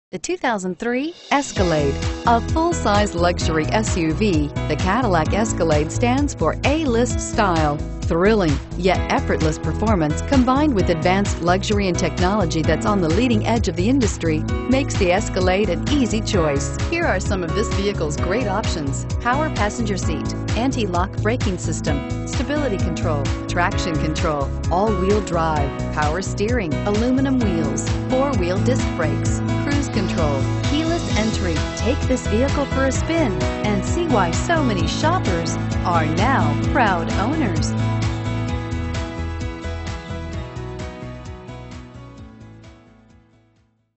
2003 CADILLAC ESCALADE Carmel, IN 5P4126B
2003 CADILLAC ESCALADE Carmel, IN
Stock# 5P4126B

You won't find a better SUV than this great 2003 Cadillac Escalade. Score this wonderful Escalade at an outstanding price that you can easily afford! J.D. Power and Associates gave the 2003 Escalade 5 out of 5 Power Circles for Overall Initial Quality Design. It is nicely equipped with features such as AWD.